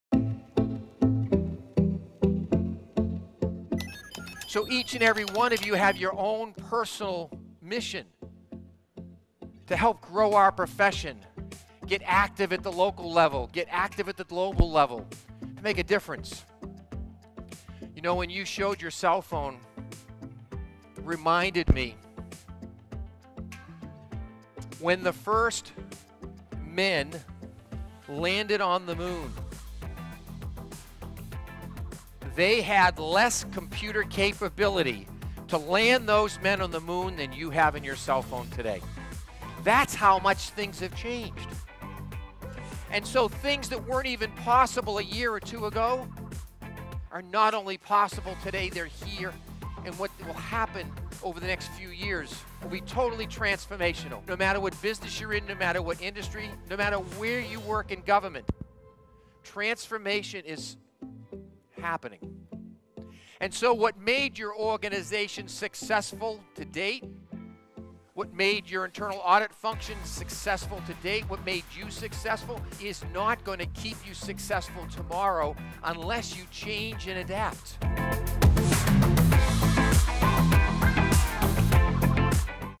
O que rolou no CONBRAI 2019 - Larry Harrington
Veja o que rolou no 39° Congresso Brasileiro de Auditoria Interna - CONBRAI, na palestra do Larry Harrington